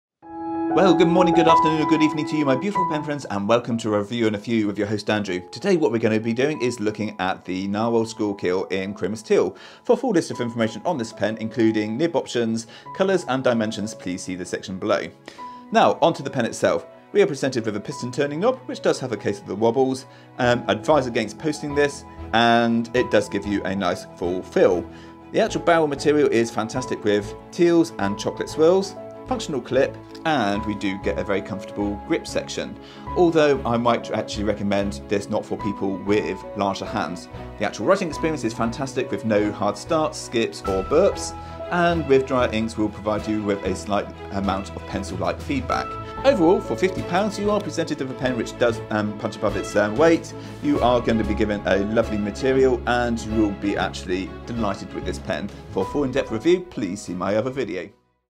Narwhal Schuylkill Chromis Teal Review
Colours - Marlin Blue, Asfur Bronze, Chromis Teal, Rockfish Red
Nib - Steel Fine and Medium
Dimensions -  144mm long capped, 132mm open, 175mm posted; weighs 24g.